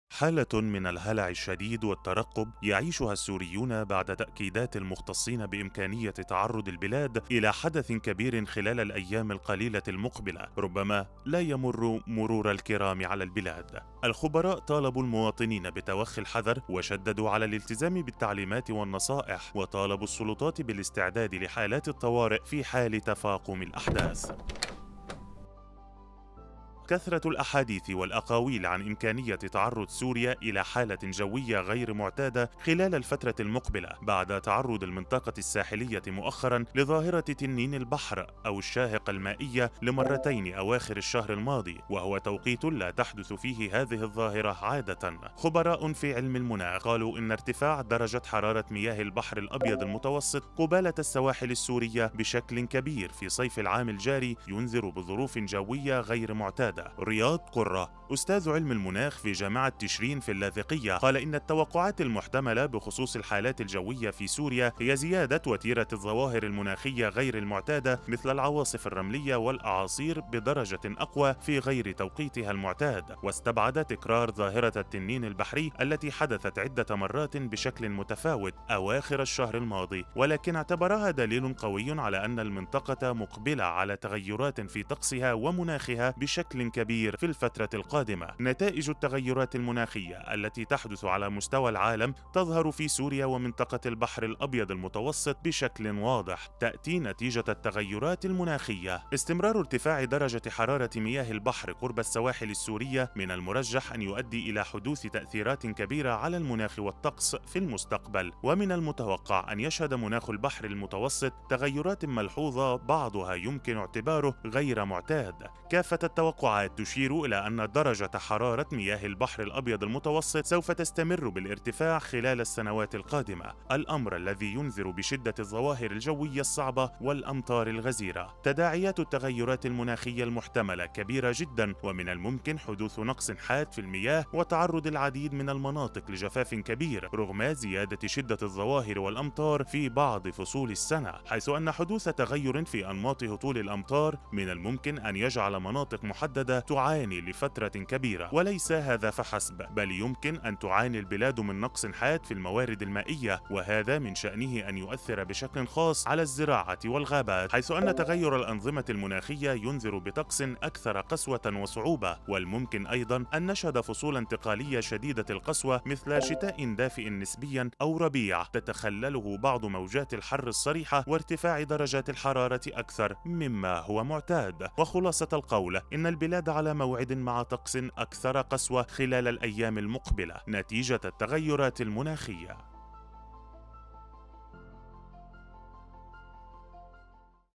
لا تغادروا البيوت أبدا.هل سيخرج تنين البحر على السوريين مرة أخرى؟هلع كبير بين السكان والسلطات تترقب
كثرة الأحاديث والأقاويل عن إمكانية تعرض سوريا إلى حالة جوية غير معتادة خلال الفترة المقبلة، بعد تعرض المنطقة الساحلية مؤخراً لظاهرة تنين البحر أو الشاهقة المائية لمرتين أواخر الشهر الماضي، وهو توقيت لا تحدث فيه هذه الظاهرة عادةً.